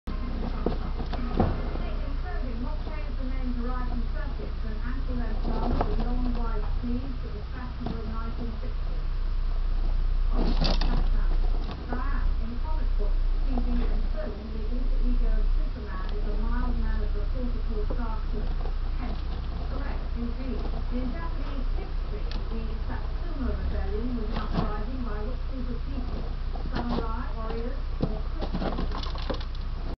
2 fighting young rats
my 2  young rats playfighting